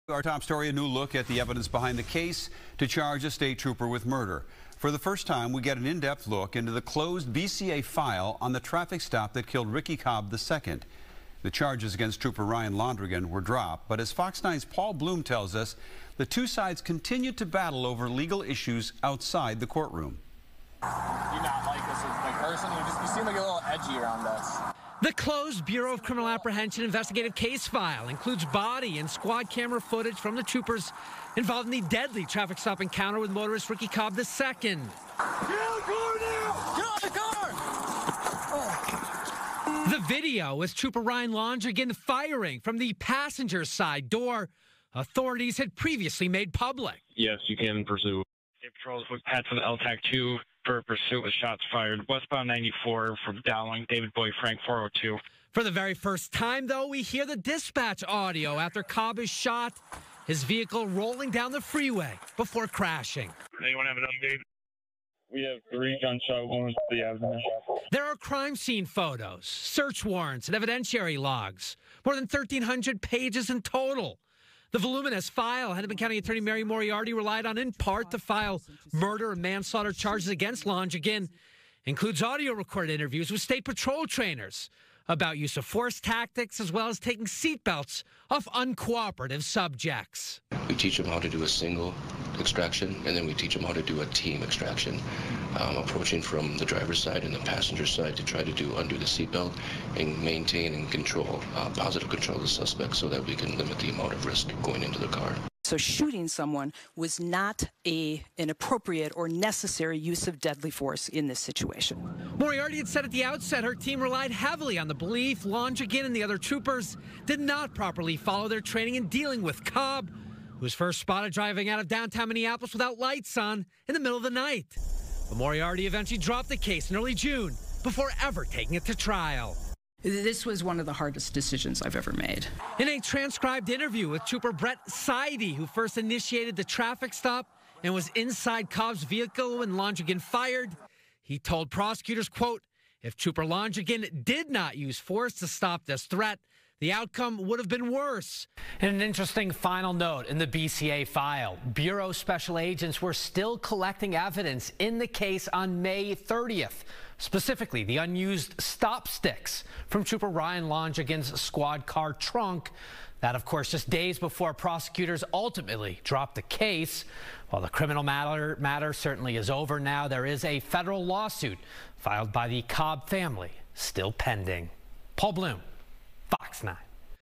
Minnesota State Trooper Londregan's case file released
Two months after the Hennepin County Attorney’s Office dropped the high-stakes case over the shooting of Ricky Cobb II by Minnesota State Trooper Ryan Londregan before ever going to trial, FOX 9 confirmed Monday that the state’s largest police organization is readying a professional conduct complaint against County Attorney Mary Moriarty for how she handled the prosecution.